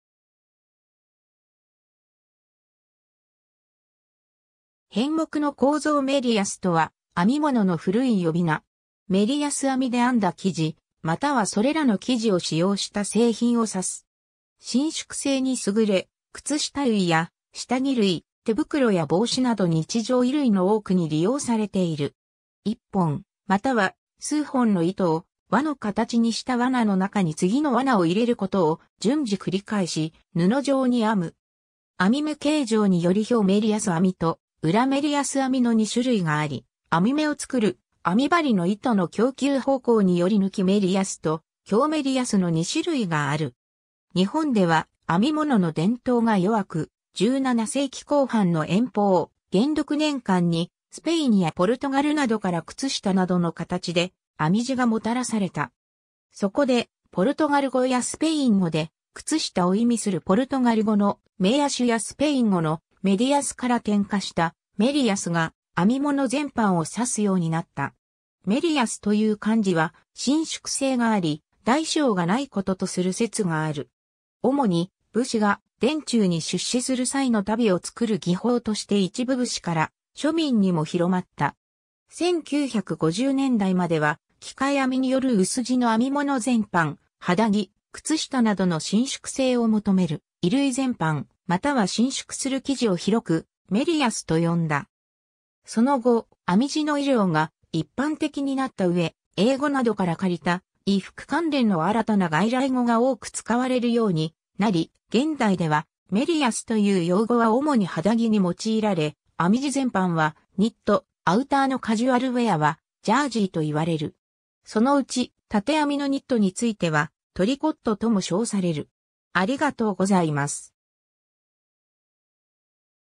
メリヤス
編目の構造 メリヤス（莫大小・目利安）とは、編み物（ニット）の古い呼び名。
 メリヤス編み（平編み・天竺編み）で編んだ生地、またはそれらの生地を使用した製品を指す。
伸縮性に優れ、靴下類や下着類、手袋や帽子など日常衣類の多くに利用されている。
 1本または数本の糸を輪の形にした輪奈（わな、loop）の中に次の輪奈を入れることを順次繰り返し、布状に編む。
編み目形状により表メリヤス編みと裏メリヤス編みの2種類があり、編み目を作る編み針の糸の供給方向により緯メリヤス（横メリヤス）と経メリヤス（縦メリヤス）の2種類がある。
 日本では編み物の伝統が弱く、17世紀後半の延宝 - 元禄年間（1673年 - 1704年）に、スペインやポルトガルなどから靴下などの形で編地がもたらされた。
そこで、ポルトガル語やスペイン語で「靴下」を意味するポルトガル語の「メイアシュ」（meias）やスペイン語の「メディアス」（medias）から転訛した「メリヤス」が、編み物全般を指すようになった。
「莫大小」という漢字は、伸縮性があり「大小がない」こととする説がある。
主に、武士が殿中に出仕する際の足袋を作る技法として一部武士から庶民にも広まった。
 1950年代までは、機械編みによる薄地の編物全般、肌着・靴下などの伸縮性を求める衣類全般、または伸縮する生地を広く「メリヤス」と呼んだ。
その後、編地の衣料が一般的になったうえ、英語などから借りた衣服関連の新たな外来語が多く使われるようになり、現代ではメリヤス（編み）という用語は主に肌着に用いられ、編地全般はニット、アウターのカジュアルウェアはジャージーと言われる。
そのうち、経編のニットについては「トリコット」とも称される。